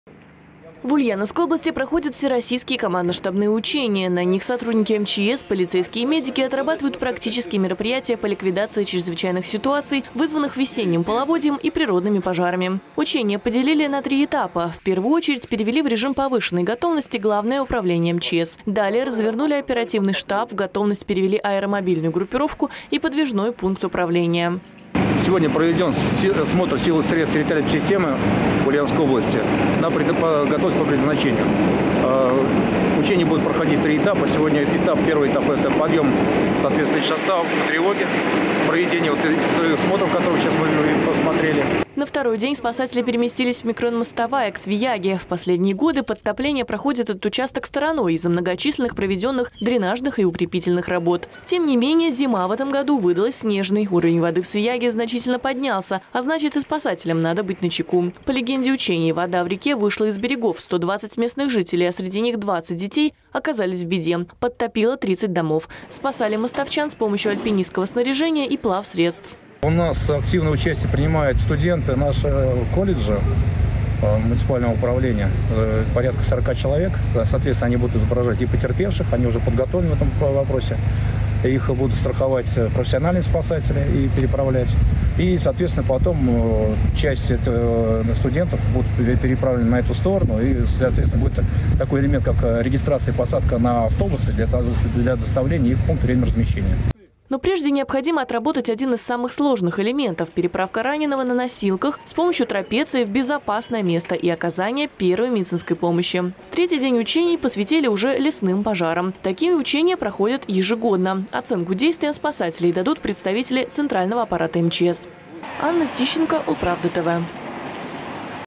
В Ульяновске учились спасать жителей затопленных домов
Весной по всей стране проходят командно-штабные учения сил МЧС России. Вместе с другими силовыми структурами и медиками они отрабатывают слаженные действия в случае внештатных ситуаций.